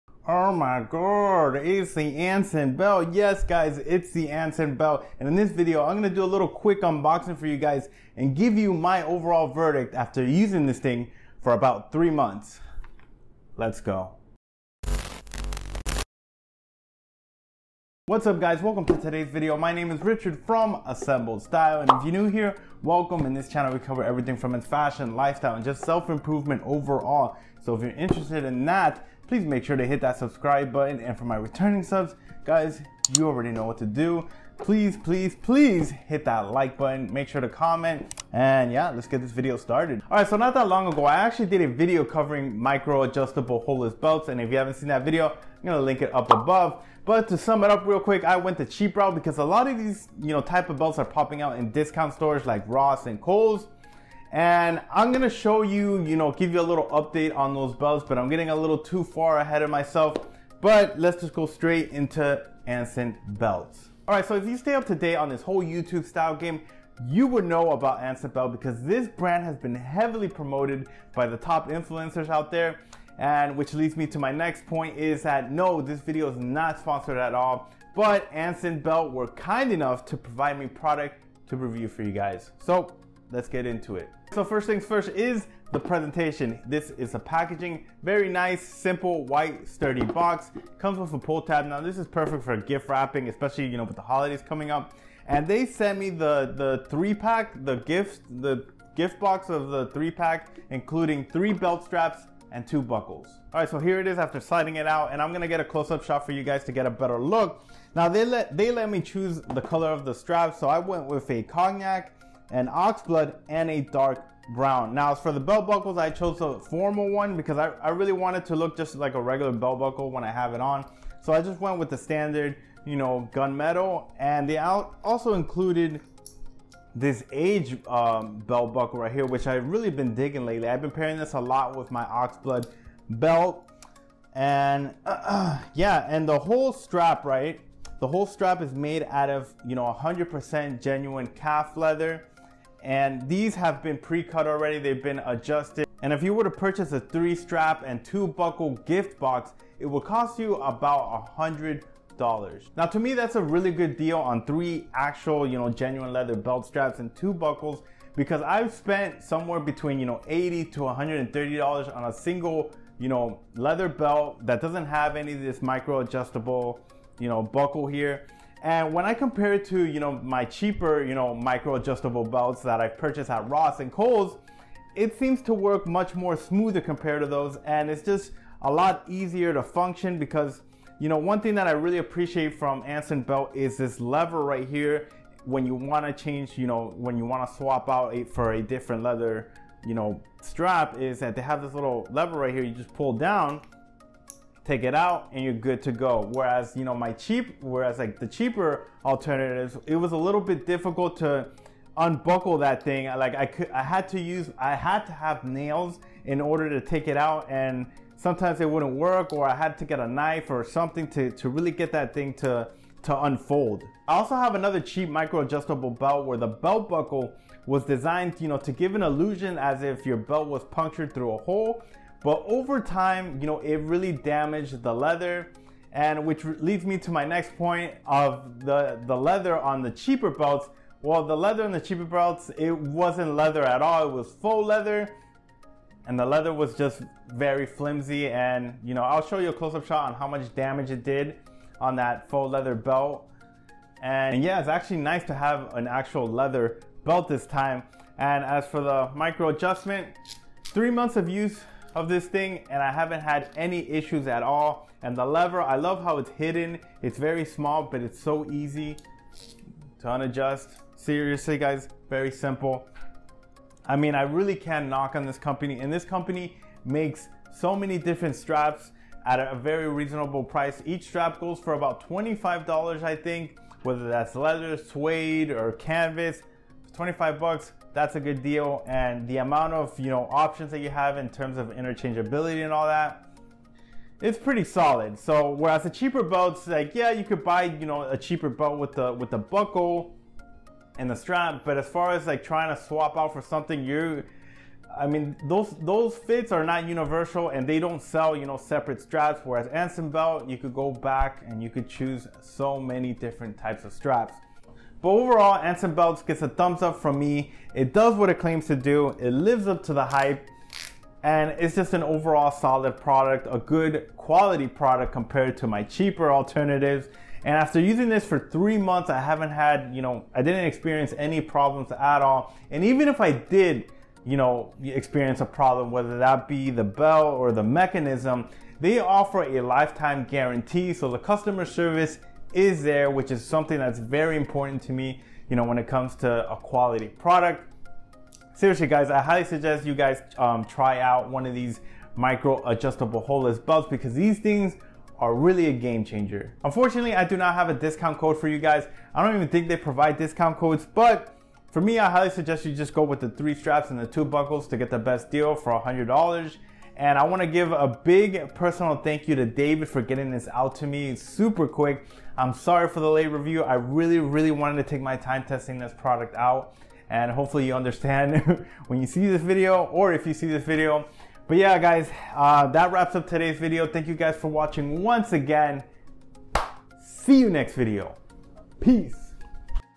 Anson Belt Buckle Unboxing and Review
In this video, I review one of the most heavily promoted products in the youtube style community named Anson belts. I have covered micro-adjustable belts in one of my past videos and tried out cheaper alternatives to see what the big deal is all about. I'm excited to finally give you guys my overall verdict on this company after testing their product for 3-months.

Purchase at Ansonbelt.com